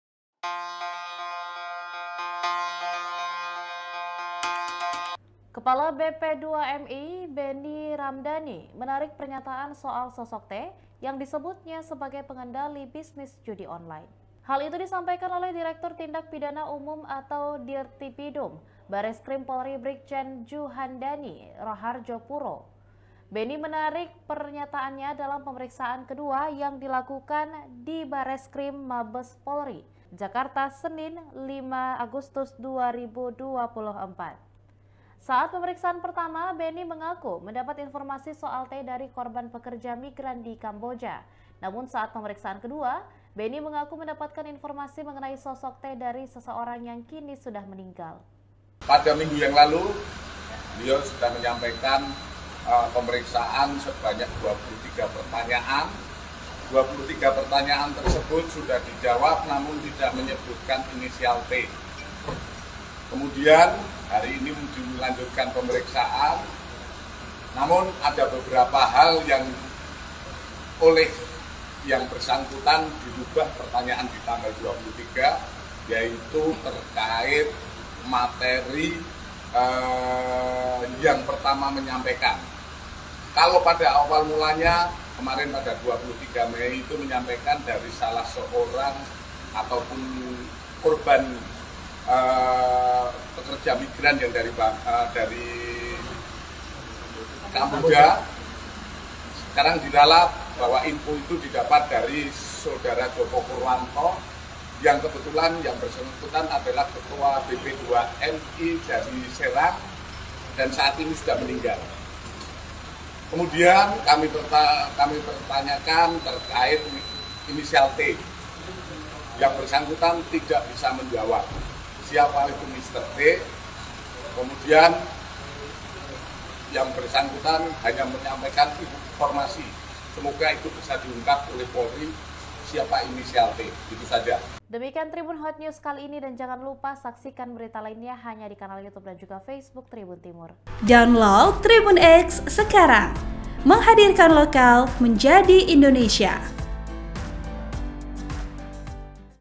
Respons Polri soal Pemeriksaan Benny Rhamdani Terkait Sosok T Pengendali Judi Online di Indonesia
TRIBUN-TIMUR.COM - Kepala BP2MI Benny Rhamdani menarik pernyataan soal sosok T yang disebutnya sebagai pengendali bisnis judi online.

Hal itu disampaikan oleh Direktur Tindak Pidana Umum (Dirtipidum) Bareskrim Polri Brigjen Djuhandhani Rahardjo Puro.

Benny menarik pernyataannya dalam pemeriksaan kedua yang dilakukan di Bareskrim Mabes Polri, Jakarta, Senin (5/8/2024).

Saat pemeriksaan pertama, Benny mengaku mendapat informasi soal T dari korban pekerja migran di Kamboja.

Namun saat pemeriksaan kedua, Benny mengaku mendapatkan informasi mengenai sosok T dari seseorang yang kini sudah meninggal.

“Karena yang bersangkutan yang menyampaikan inisial T itu ternyata juga tidak ada. Dari mana (sumbernya)? Dari Pak Joko Purwanto, Pak Joko Purwanto saat ini sudah meninggal,” kata Djuhandhani di Mabes Polri, Jakarta, Senin malam.

Program: Tribun Hotnews
Host: Annisa Husnuzhan
Editor Video: Asriadi
Kameramen: Multazam Firdaus

(TRIBUN-TIMUR.COM)